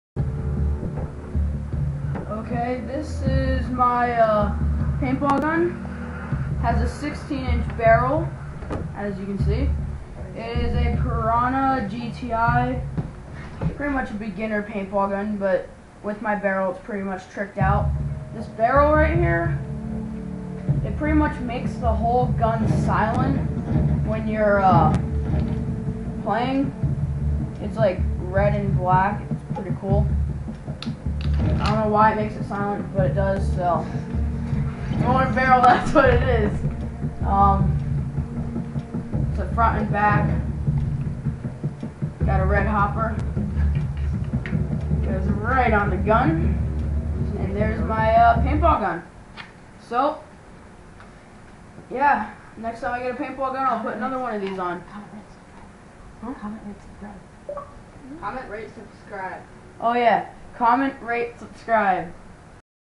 pirana paintball gun
with my customized extended barral. gun = 112$ and barrel=80$